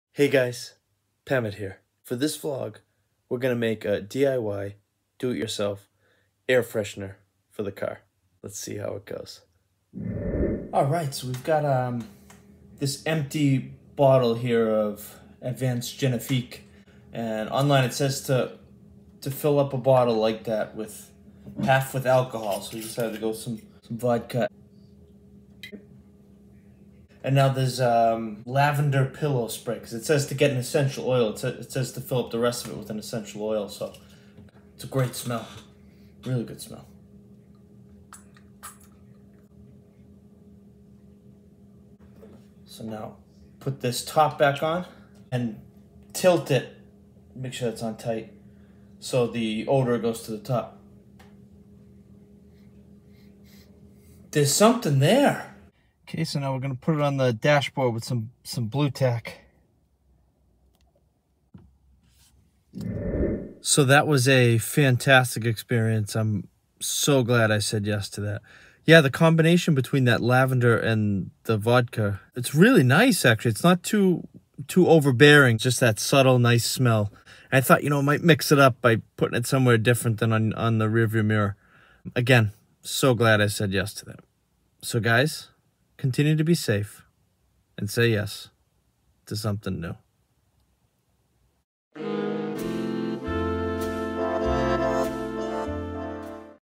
DIY Car Air Freshener - Yes to New Vlogs
I said yes to making a car air freshener from alcohol and lavender.